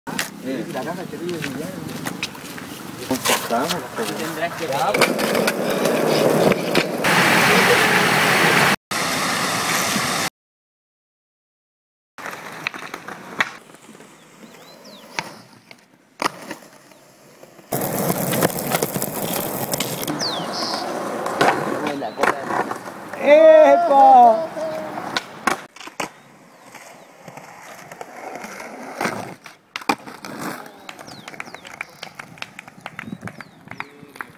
Skate de Domingo / Sebastian Bastias, Sergio Aros y Dario Covarrubias
Patin de dia domingo con Home Skateboards y Disobey Wheels. Sebastian Bastias, Sergio Aros y Dario Covarrubias por las cercanias de Quilpue.